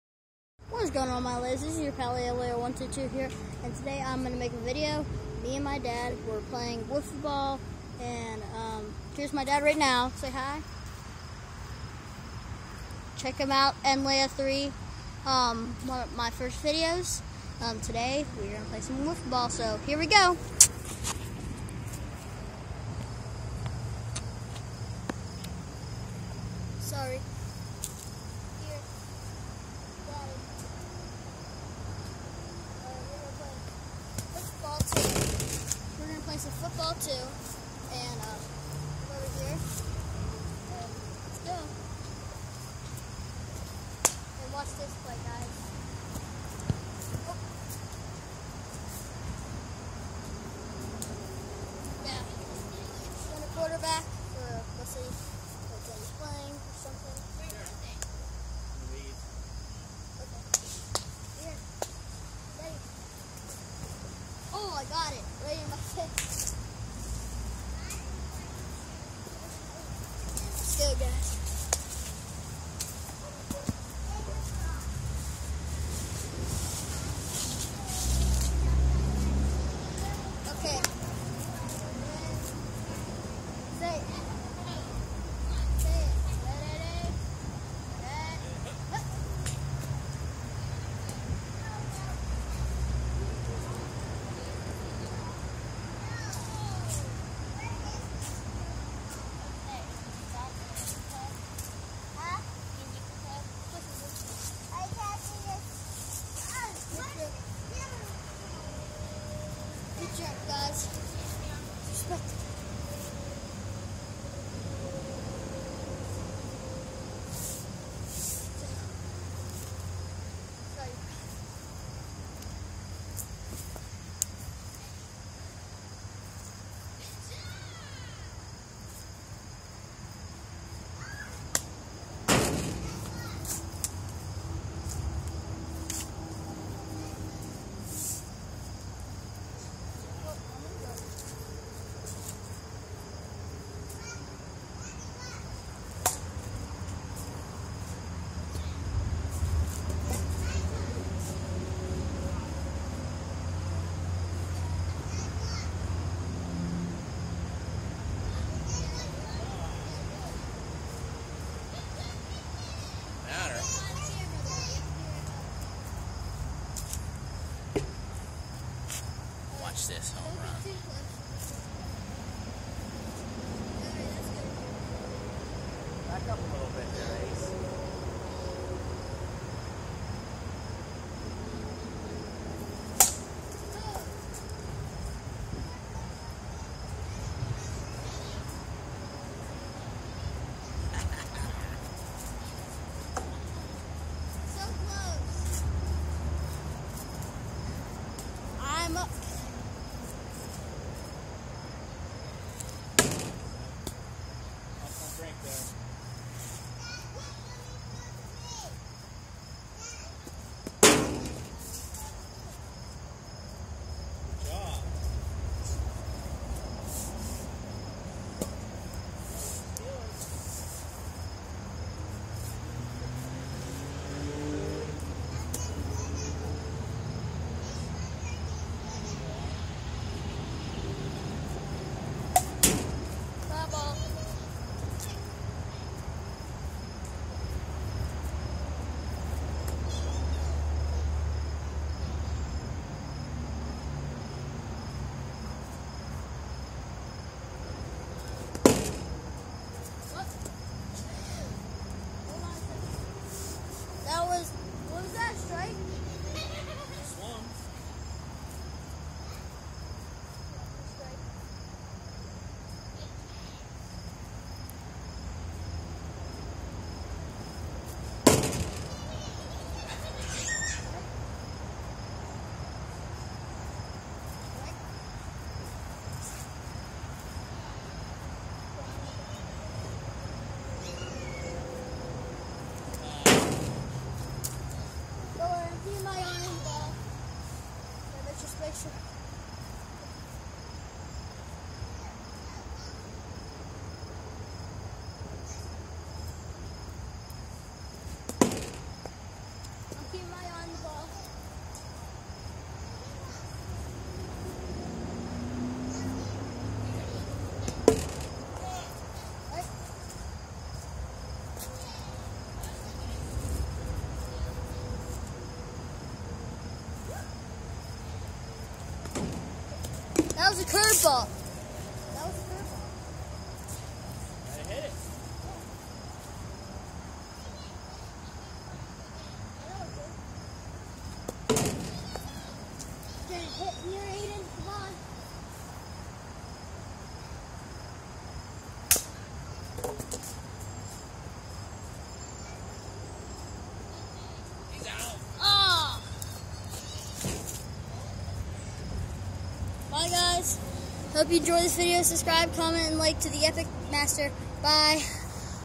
Playing wiffleball and football with my father
Really good day with my father